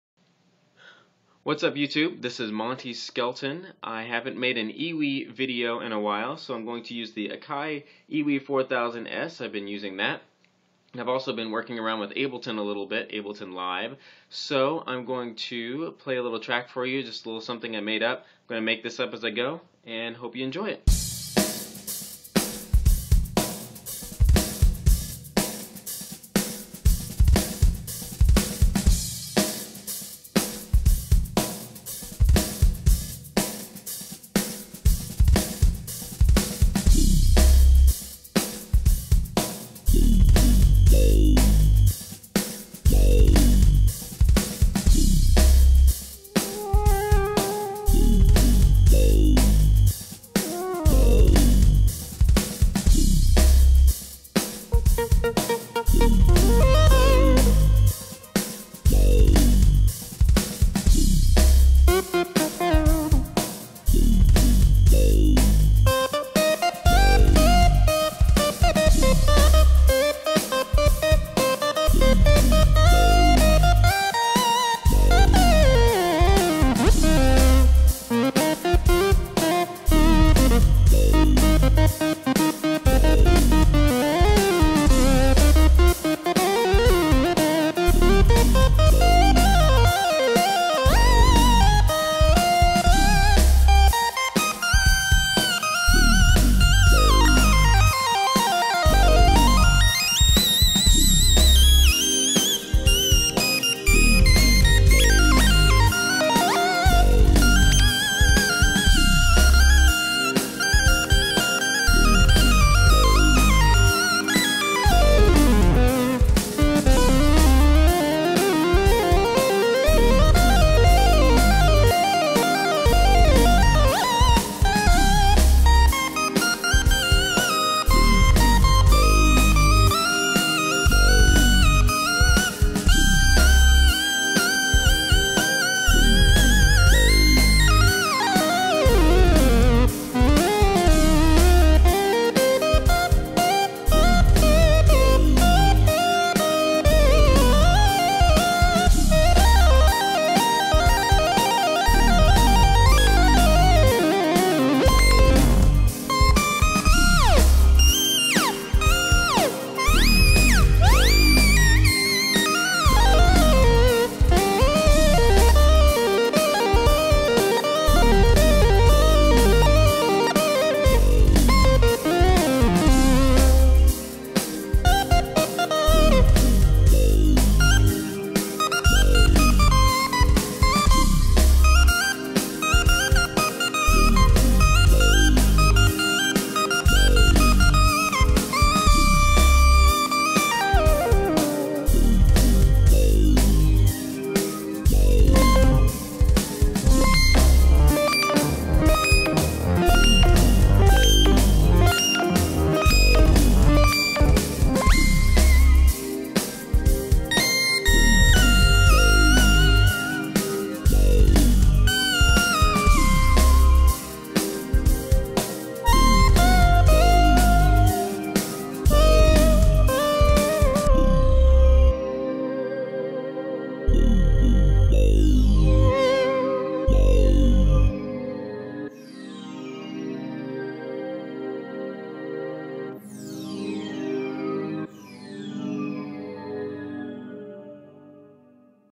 Monte Skelton- EWI Jam
I just thought I would add a new EWI video since I haven't uploaded one in forever!  I hope you like it!  I used Ableton Live to make the background music, An Operator for the bass, just in case you're wondering.  I'm using the Akai EWI 4000s to create this video.  Let me know what you think, and if you have any questions, let me know!  Thanks!